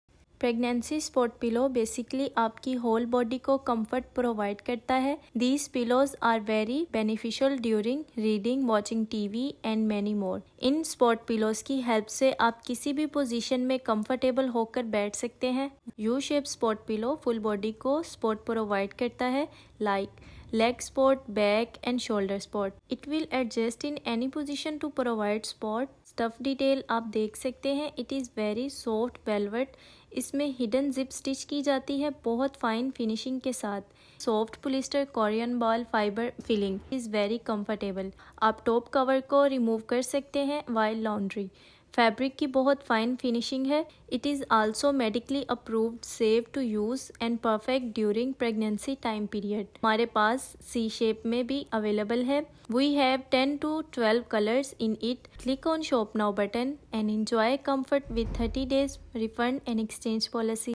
𝗛𝗼𝘁 𝗦𝗲𝗹𝗹𝗶𝗻𝗴 𝗨-𝗦𝗵𝗮𝗽𝗲 𝗦𝘂𝗽𝗽𝗼𝗿𝘁 𝗣𝗶𝗹𝗹𝗼𝘄𝘀 || 𝗩𝗲𝗹𝘃𝗲𝘁 𝗠𝗮𝘁𝗲𝗿𝗻𝗶𝘁𝘆 𝗣𝗶𝗹𝗹𝗼𝘄𝘀
This Support Pillow Specifically Designed To Support The Changing Curves Of Your Body Throughout Your Pregnancy. A Good Pillow Can Make All The Difference, This Is Important To Have For Ultimate Comfort, Pain Relieve & Body Support.

𝗗𝗲𝘁𝗮𝗶𝗹𝘀:
◼ Fine Velvet Stuff.
◼ Imported Korean Ball Fibre Filling.
◼ Hot Selling & Top Rated ⭐⭐⭐⭐⭐ Product.
◼ Cover Is Removable & Washable.
◼ 8 To 9 Colors Are Available.
◼ 30 Days Refund & Exchange Policy.
𝗜𝘁 𝗪𝗶𝗹𝗹 𝗣𝗿𝗼𝘃𝗶𝗱𝗲 𝗬𝗼𝘂:
◼ Head & Neck Support.
◼ Back, Lower Back & Belly Support.
◼ Knee, Ankle & Foot Support.
◼ Full Seated & Elevated Support.
◼ Medically Approved & It Offers Support For Various Safe Sleeping Positions & More.